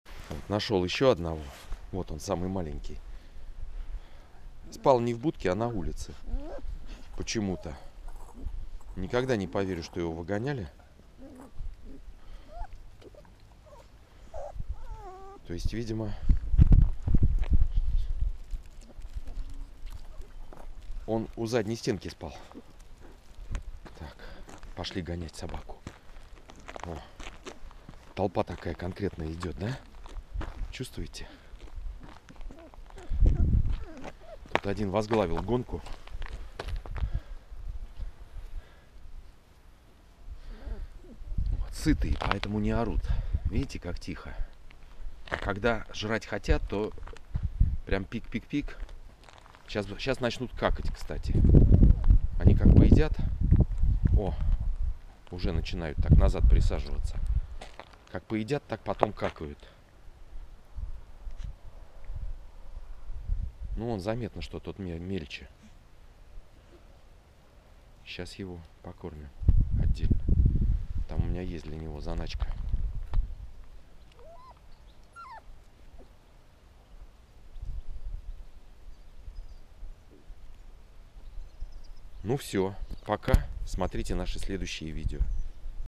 Щенки бегают по снегу
Один месяц и 6 дней. Вторая серия.